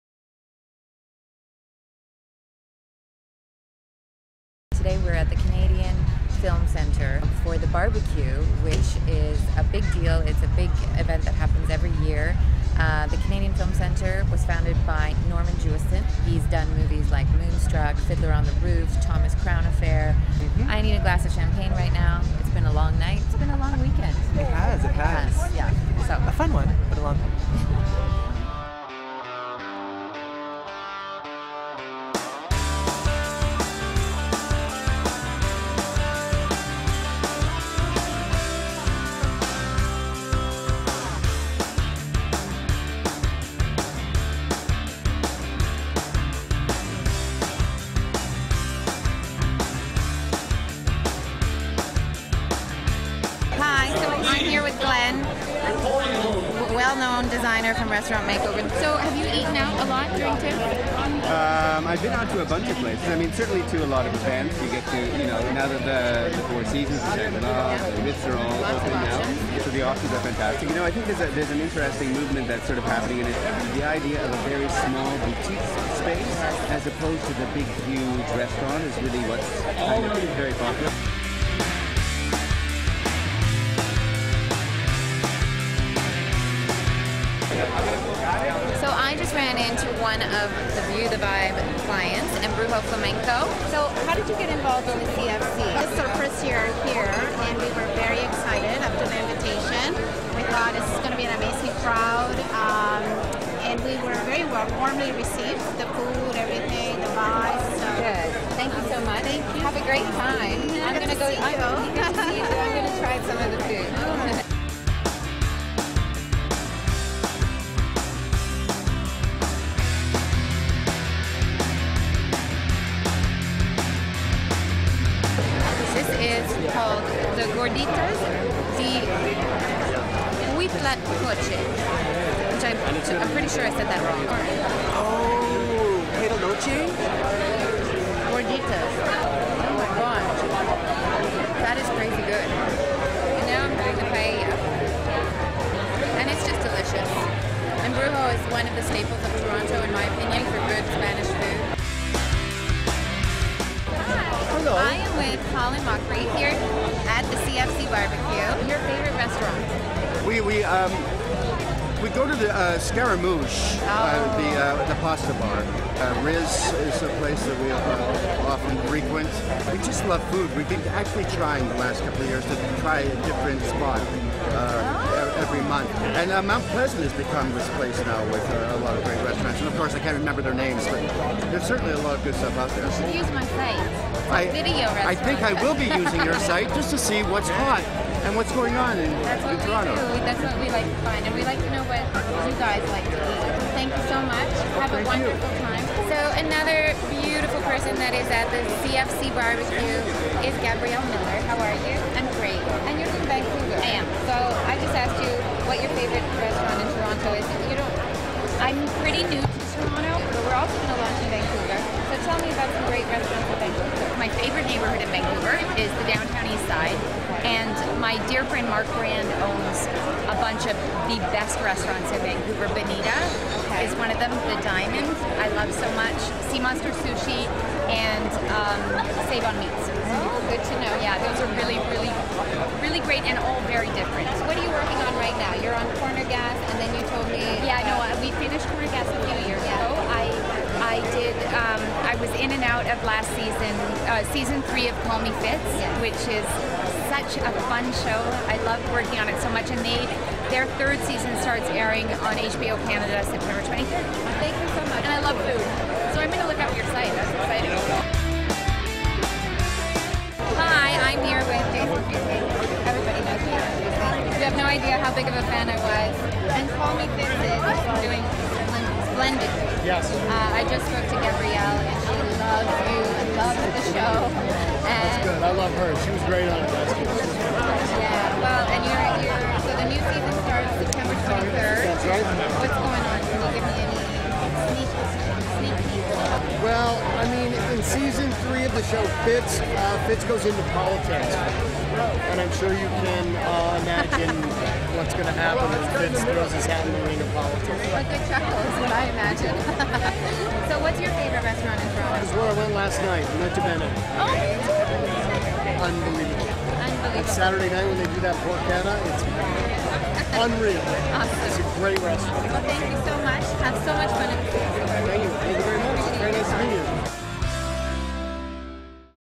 CFC BBQ
TIFF just wouldn't be TIFF without the annual Canadian Film Centre (CFC) BBQ hosted by Norman Jewison, right? Okay, in case you haven't heard of this one, I'll give you a primer...
The CFC was started in 1988 by Jewison -- you know, the director behind such classics as In The Heat of the Night, The Thomas Crown Affair (the original), Fiddler on the Roof, Jesus Christ Superstar, and Moonstruck among others -- to foster the development of the Canadian filmographic landscape, providing mentoring, education and guidance to students in most areas of film production. Since its inception, the CFC has seen over 1,000 graduates of their various programs.